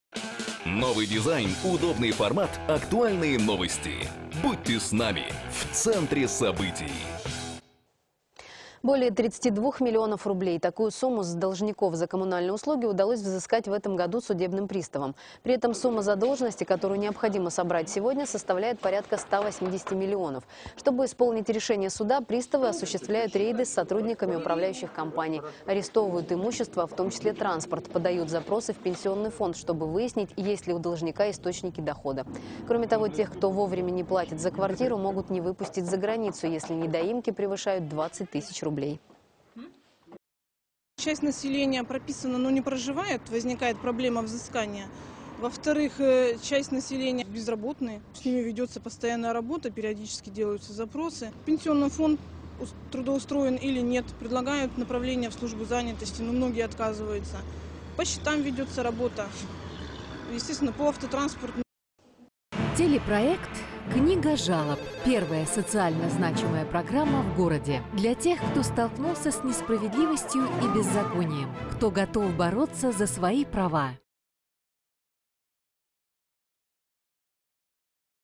Приставы (ВЦС от 15.05.14)
Более тридцати двух миллионов рублей. Такую сумму с должников за коммунальные услуги удалось взыскать в этом году судебным приставам.